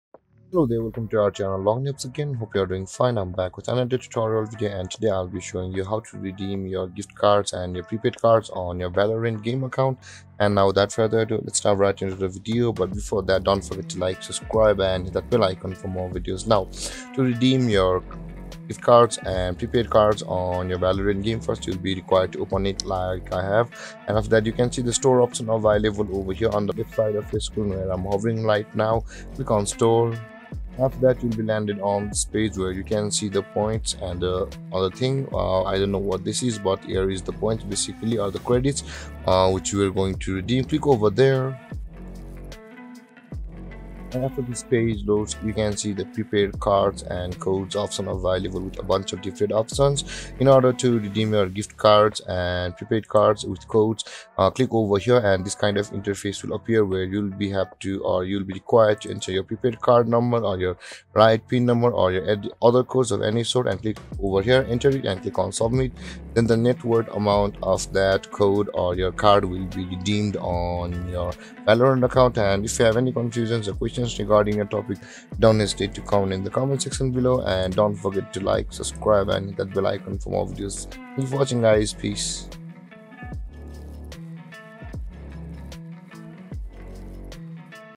How to Redeem Valorant Gift Card | Redeem Valorant Giftcard
Get ready to level up your gaming experience with this guide to redeeming your Valorant gift card. We'll show you step-by-step how to redeem your gift card and get access to new weapons, skins, and more.